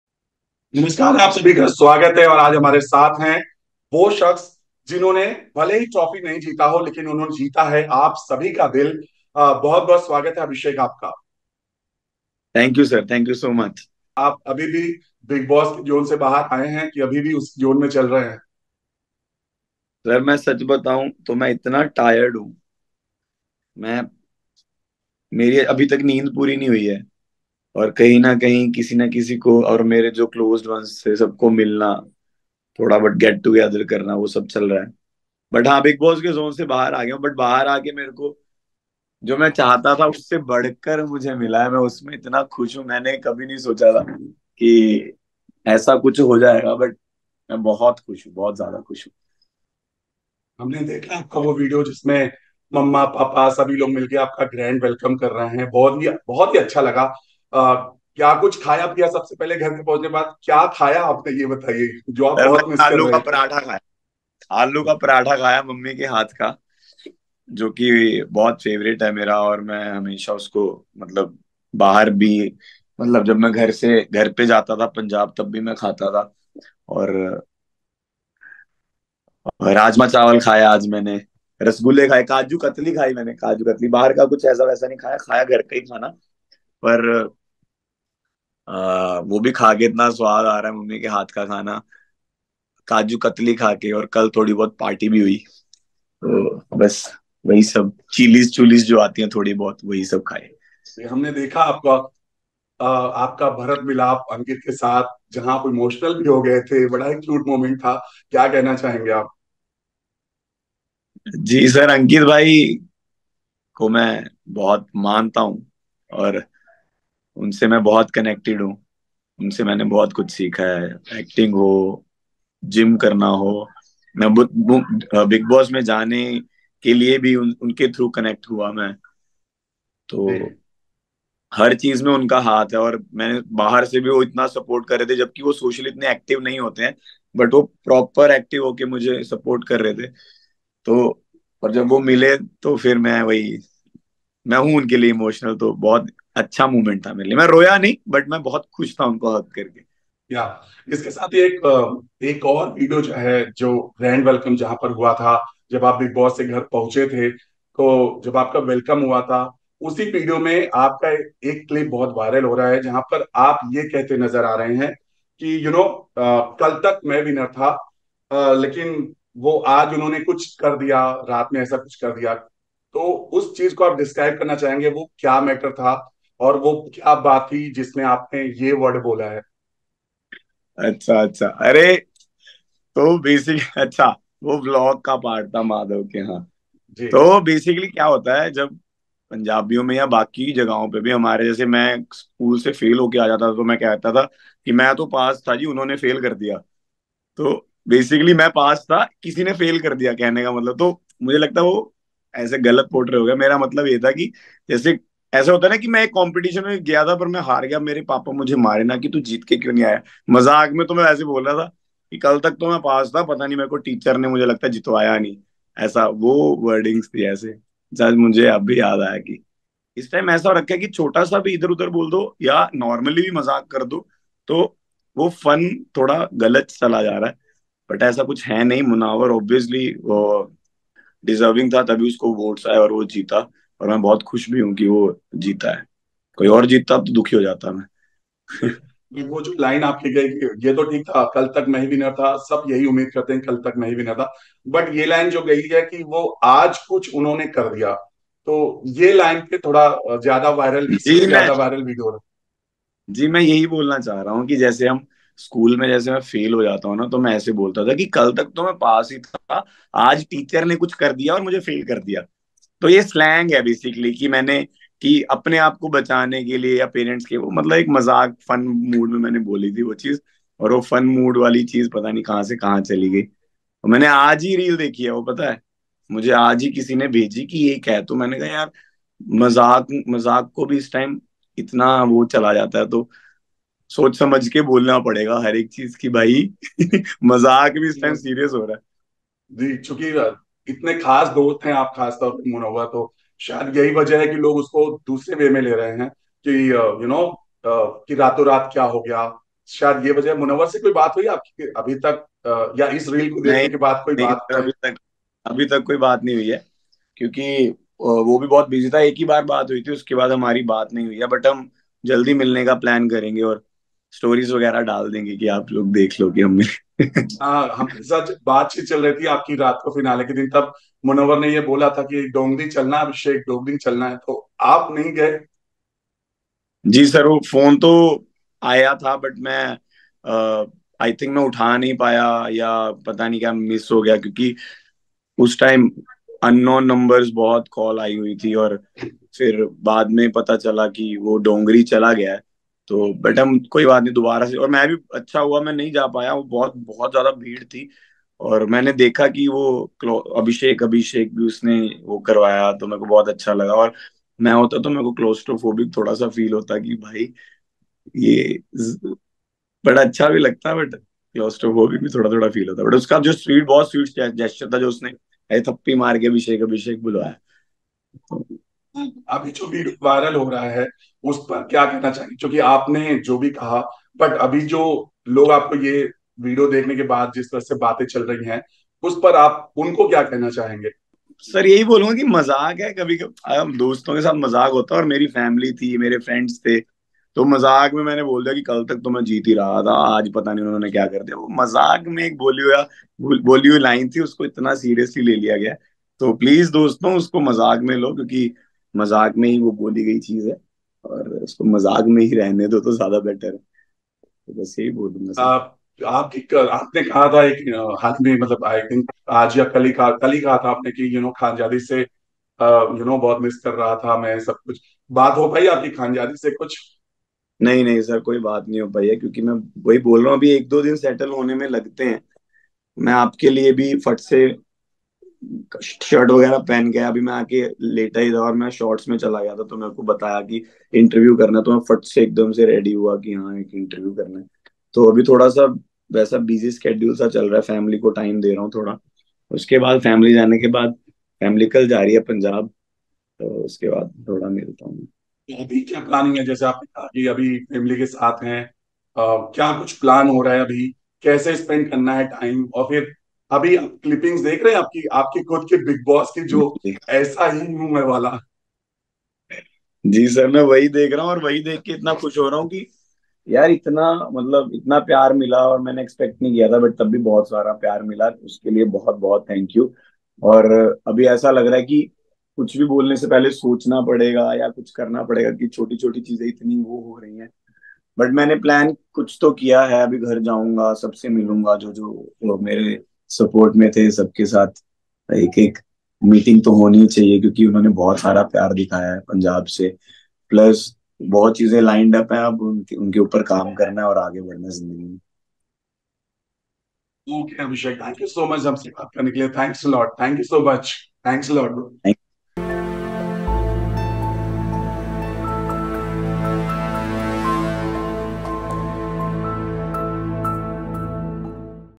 Abhishek Kumar’s EXCLUSIVE reaction on his viral video saying, “Kal tak main winner tha”
TV News – Bigg Boss 17’s first runner-up Abhishek Kumar in an exclusive interview with Telly Talk reacts to his viral video of him claiming that, “Kal tak main winner tha”. He says Munawar Faruqui is a deserving winner as he got more votes. He opens up on his bond with Ankit Gupta & partying with friends. Abhishek also reveals whether he had a chat with Khanzaadi or no. Watch the video to know more!